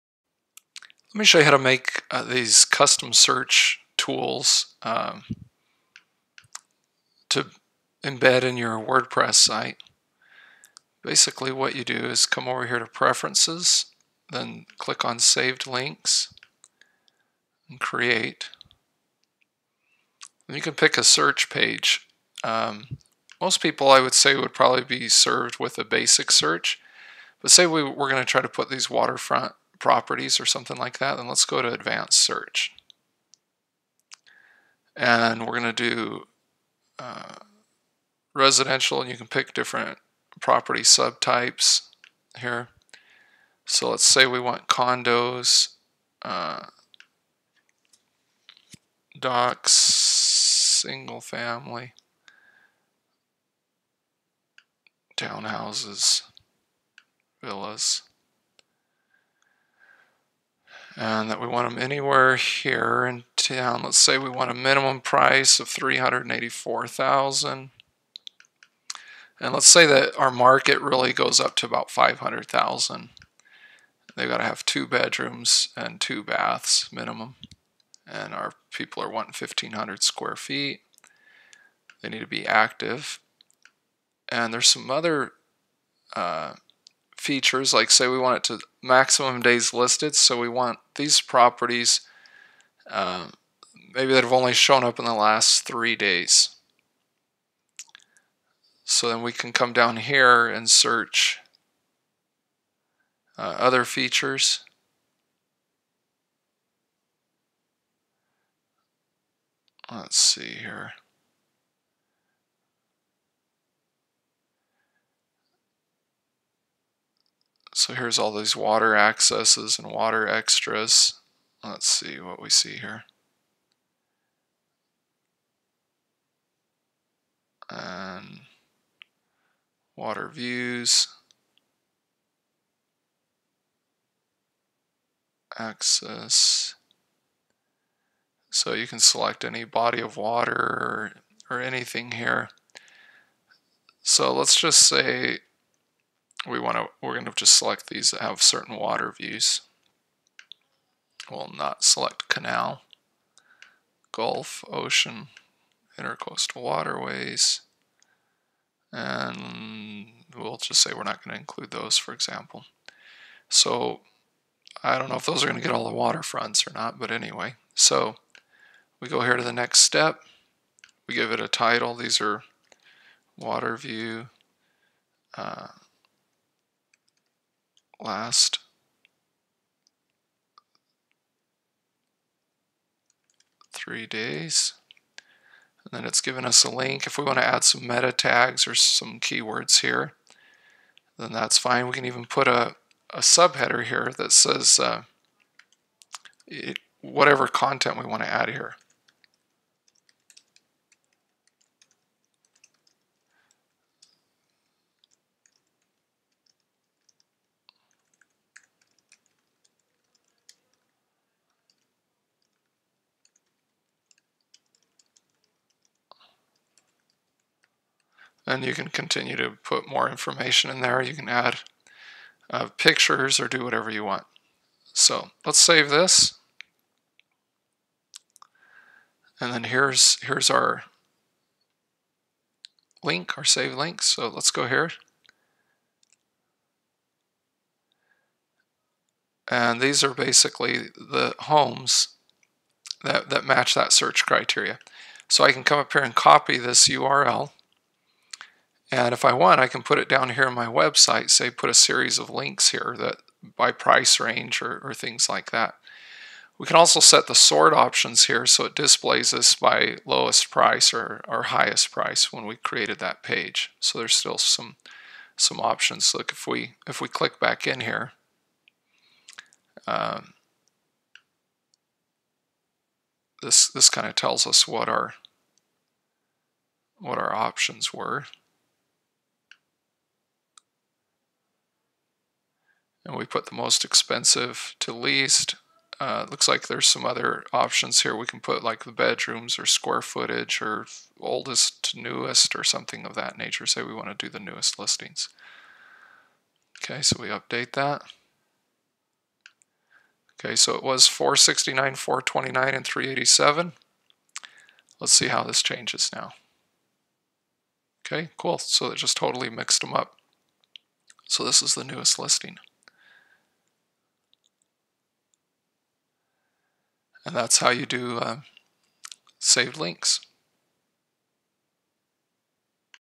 IDX Broker saved links from advanced search for WordPress
Create custom saved searches using IDX Broker Platinum advanced search options.

These searches can be saved as links to your WordPress real estate website.